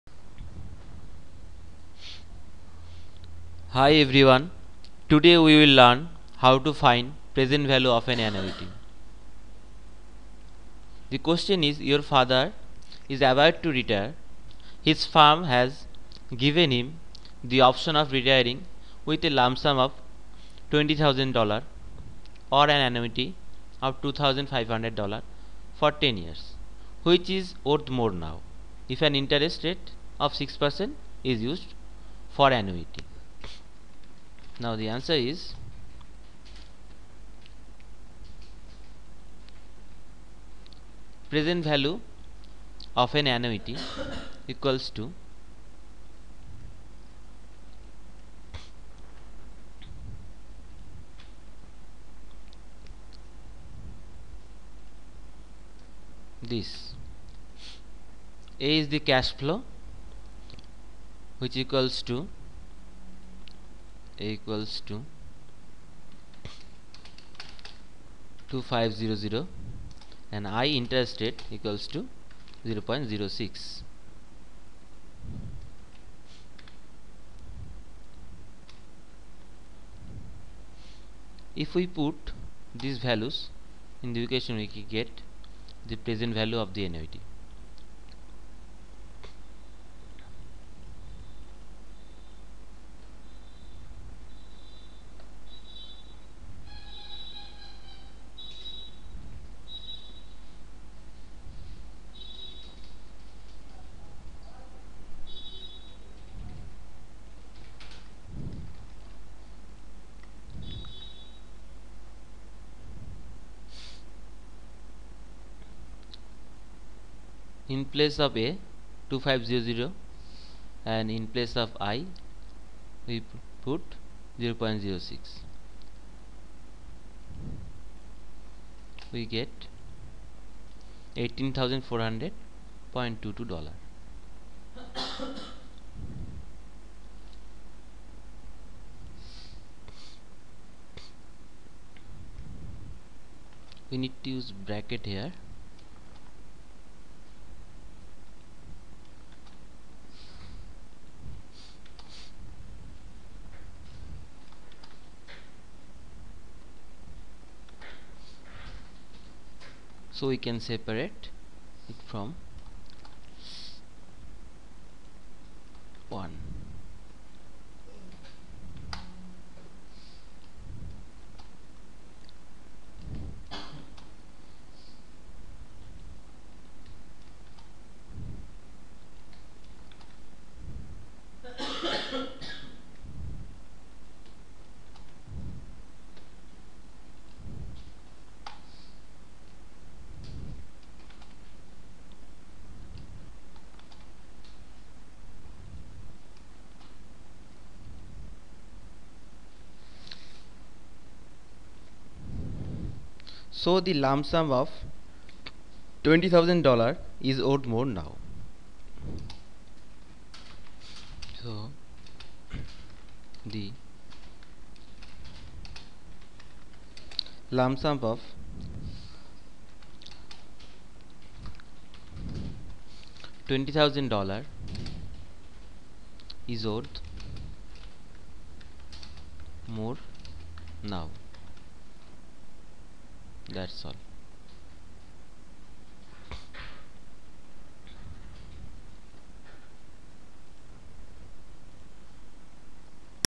Present Value of An Annuity - Accounting Help | TutorTeddy.com
for free math help .
We provide statistics help at  a low cost or even free.
Question: Your father is about to retire. His firm has given him the option of retiring with a lump sum of $20,000 or an annuity of $2,500 for 10 years. Which is worth more now, if an interest rate of 6% is used for annuity?